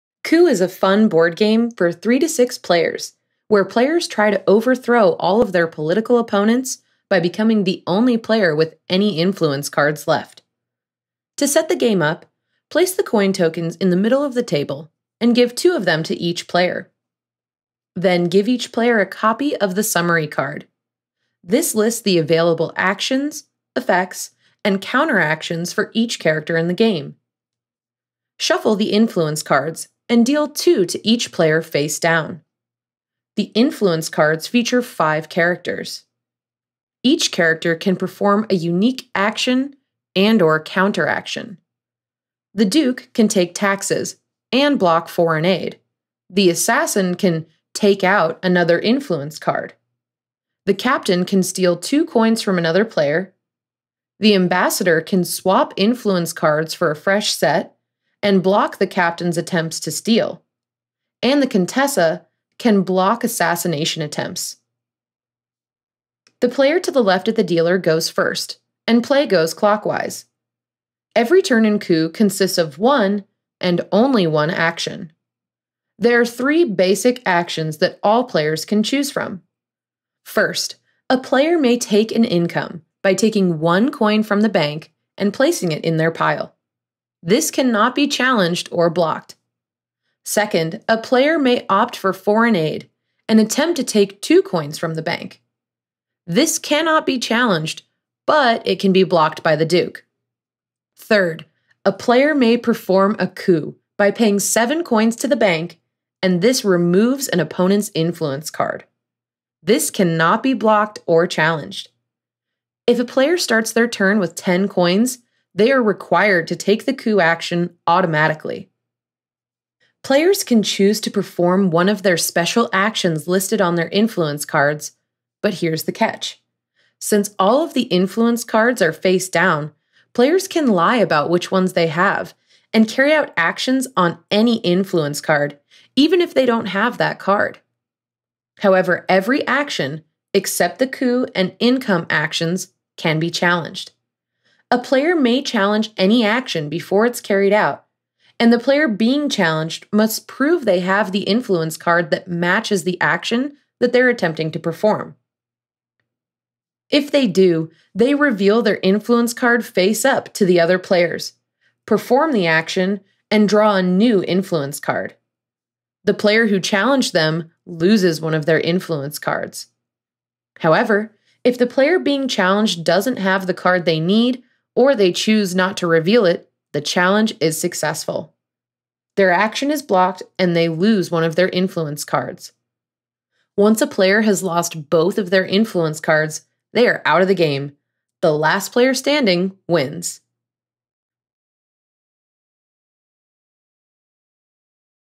How to Play Coup
Coup is a fast-paced, exciting game of domination. The goal is to eliminate the other players so you can assume absolute power! Watch our short video to learn the basics to make sure you're the last player standing. Read more about how to play Coup with this guide from wikiHow

We've included some products we think are useful for viewers.